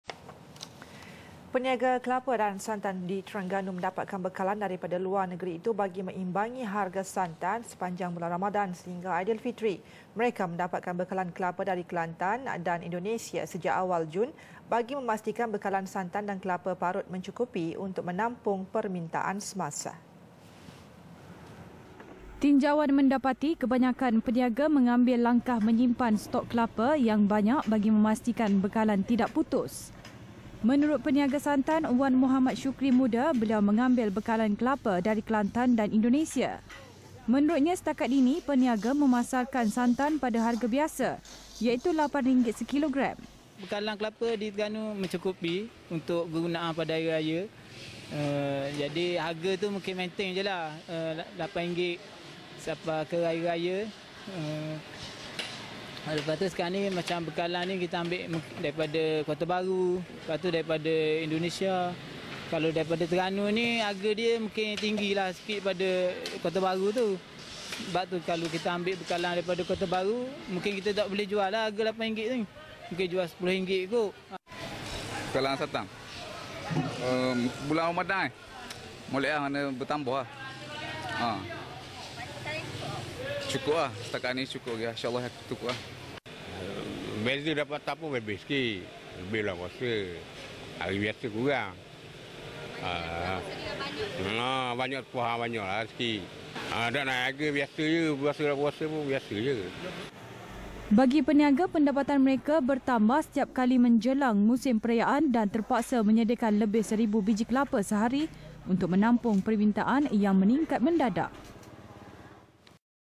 Bekalan kelapa dan santan mencukupi di Terengganu
Peniaga kelapa dan santan di Terengganu mendapatkan bekalan daripada luar negeri itu  bagi mengimbangi harga santan sepanjang bulan Ramadan sehingga Aidfilfitri.

Mereka mendapatkan bekalan kelapa dari Kelantan dan Indonesia sejak awal Jun bagi memastikan bekalan santan dan kelapa parut mencukupi untuk menampung permintaan semasa.